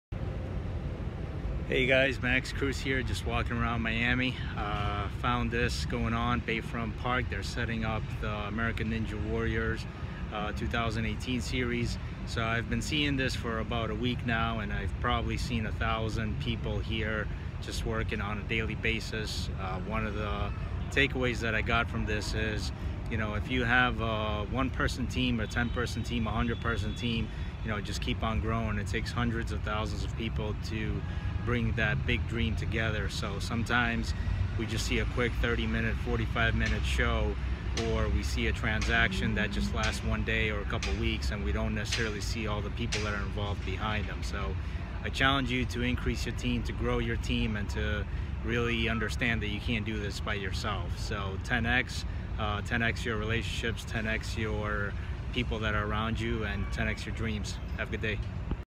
How can you build a business on your own?
You can try to build something on your own but at some point you will need additional people around you to build the best version of your idea. 10X your relationships so you can 10X your dreams 10 times faster.